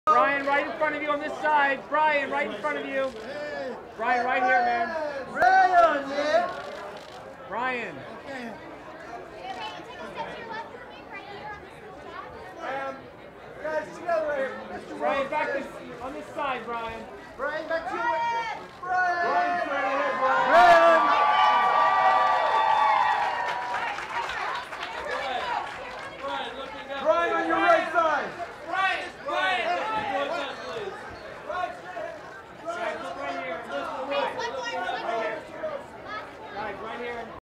Brian Wilson, Melinda Wilson at The 55th Annual GRAMMY Aw...
Brian Wilson, Melinda Wilson at The 55th Annual GRAMMY Awards - Arrivals 2/10/2013 in Los Angeles, CA.